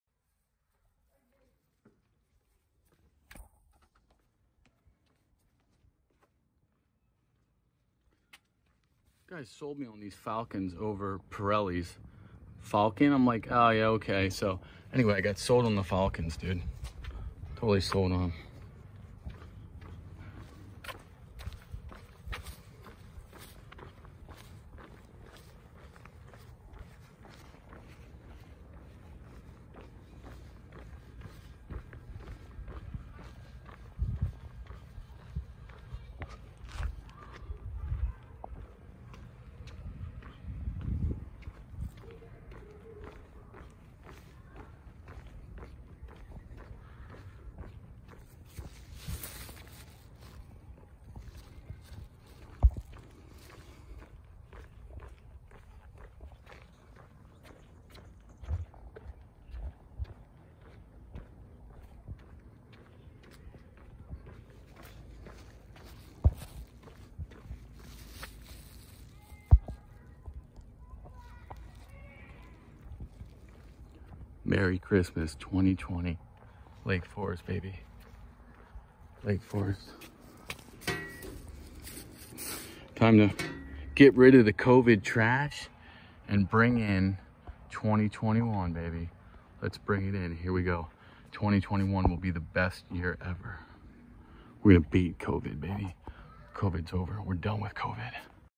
Walking COVID-19 to the trash
2020 will go down in history as the year of the Covid 19 pandemic. In this video I am walking out the dogs shit to the trash. The analogy is this dog shit is Covid.